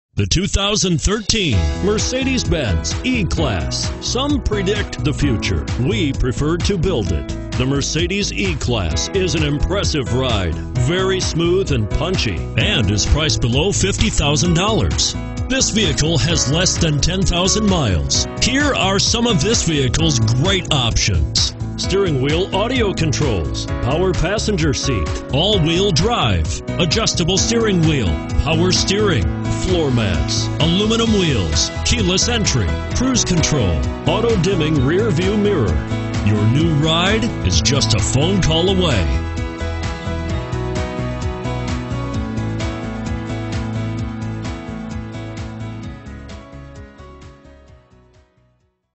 2013 MERCEDES-BENZ E-CLASS New London, CT 3274
2013 MERCEDES-BENZ E-CLASS New London, CT
Stock# 3274

Carriage House Mercedes-Benz
488 Colman St.

You'll love this Certified 2013 Mercedes-Benz E-Class. This is a car you'll want to take home. With 9640 miles, it features Automatic transmission and an exterior color of Iridium Silver Metallic. Call us and be the first to open the car door today!